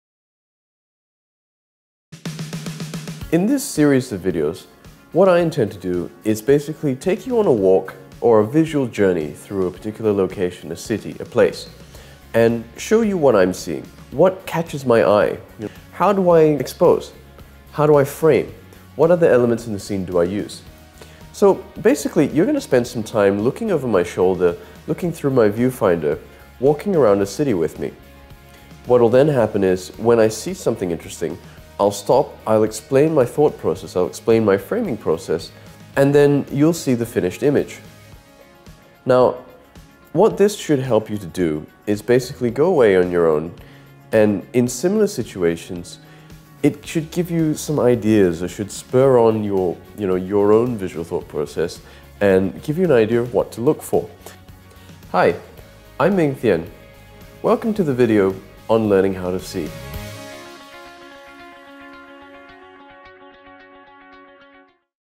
Trailer: How To See, Ep1: Kuala Lumpur
This will be a new series of videos that merges travel and photography: simply put, you spend some time with me whilst I shoot in various cities around the world; you see over my shoulder, inside my viewfinder, and of course get the benefit of discussion of the ensuing images. In essence, you get to experience vicariously how I view the world. Needless to say, we start with my home city: Kuala Lumpur. I explain what's catching my attention in a particular location, how and why I chose that particular framing and processing. On top of that, we talk about why a particular image works - or doesn't. Episode two will be shot in Tokyo, and available around the end of November.